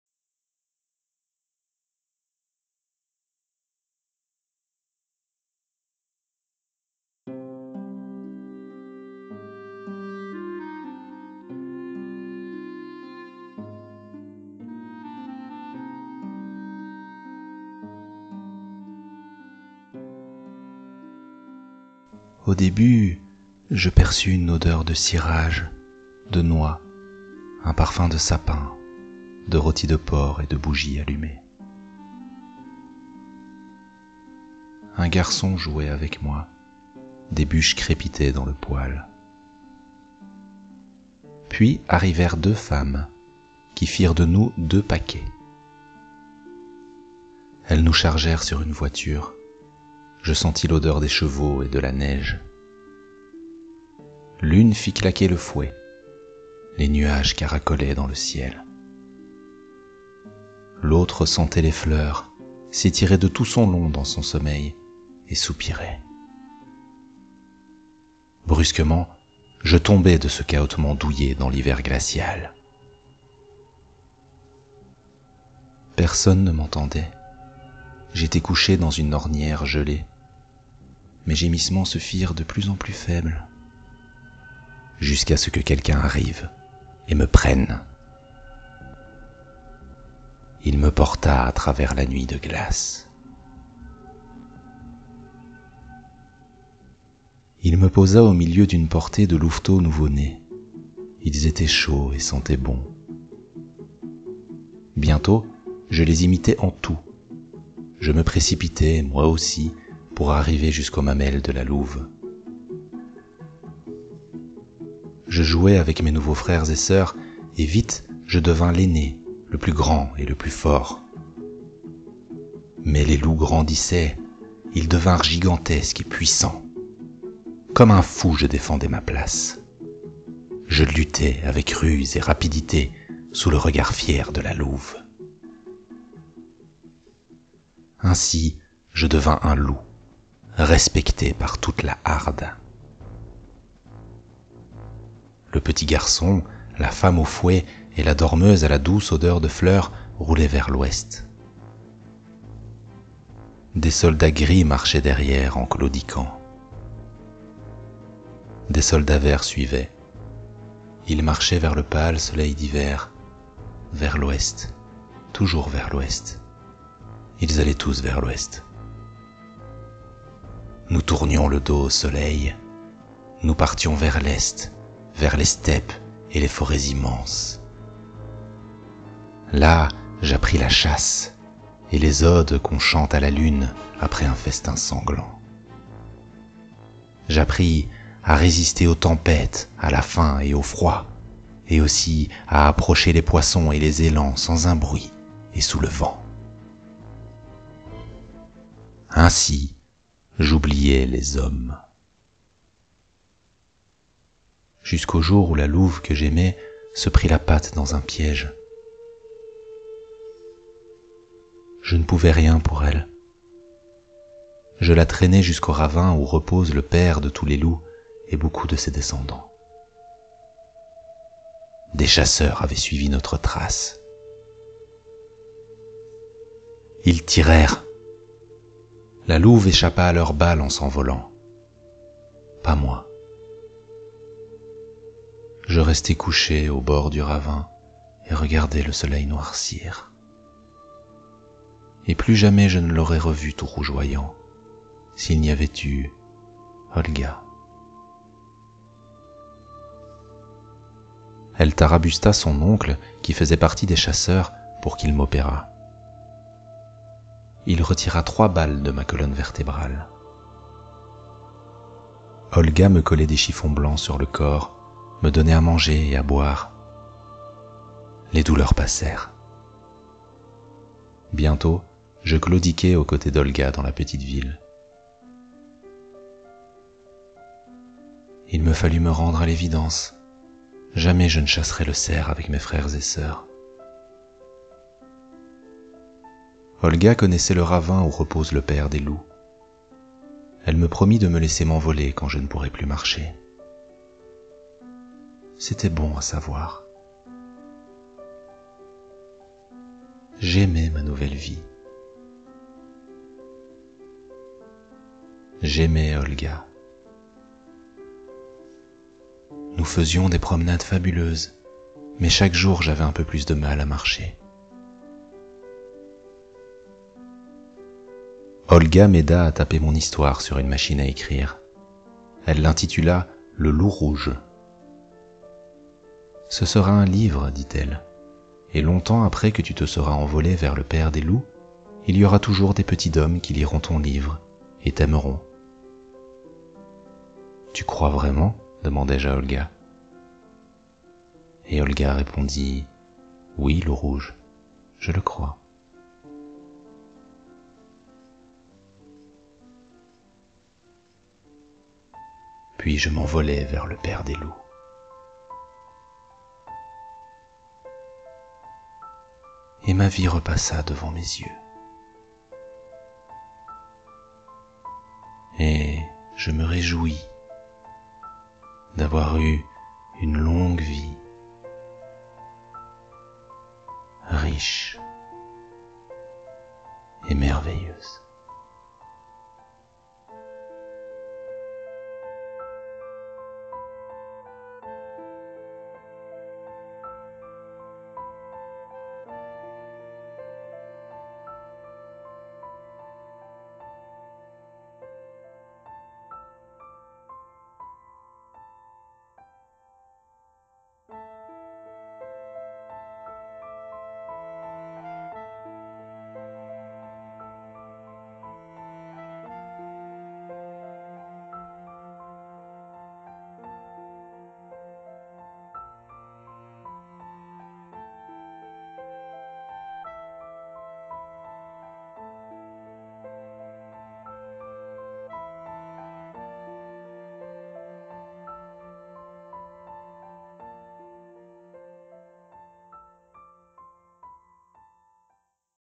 LE LOUP ROUGE lecture audio
Dans cette vidéo, je vous propose une lecture audio du récit "Le loup rouge", écrit et illustré par Friedrich Karl Waechter.

Cette histoire émouvante raconte la vie d'un petit fox terrier adopté par des loups, dans les vastes steppes glacées de l'Est.

Même s'il s'agit d'un livre pour enfants, j'ai choisi d'en faire une interprétation plus mature et mélancolique. J'espère qu'elle vous plaira.

Passez faire un tour sur mon blog ! Vous y trouverez des récits courts de plusieurs genres différents